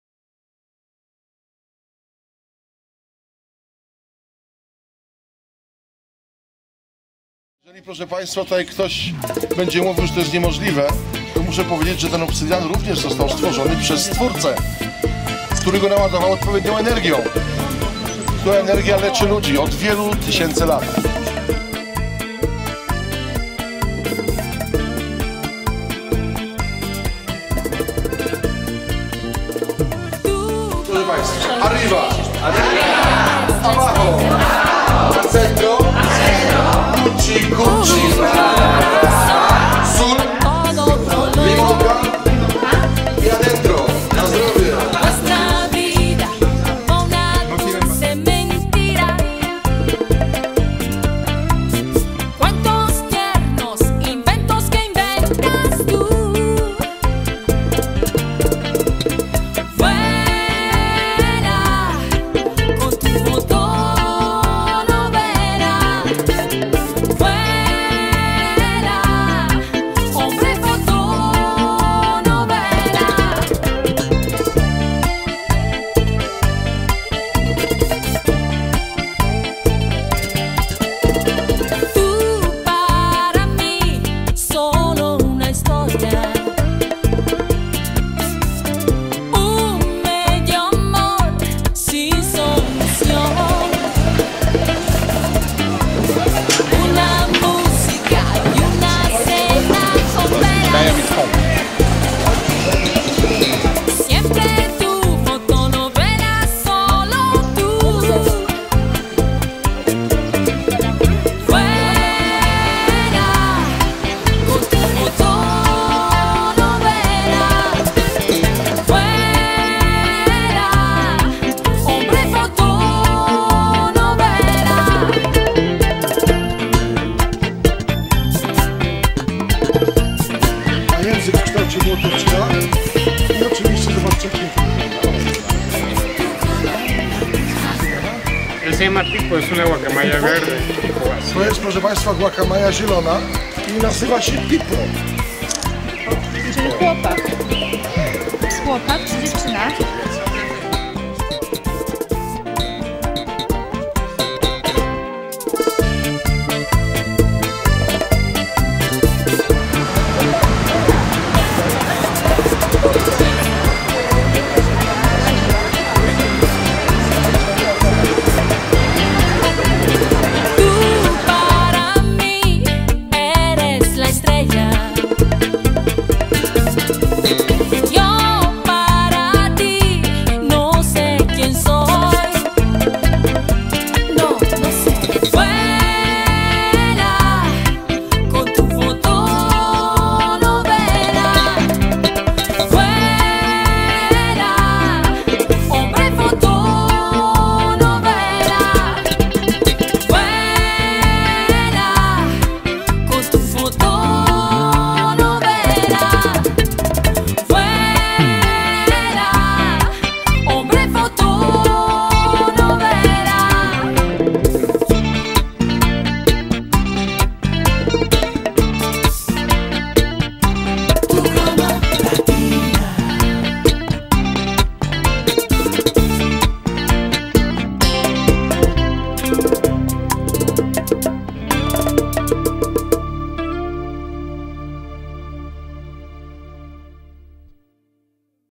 Tabasco
This video is about our trip to Mexico in April 2015.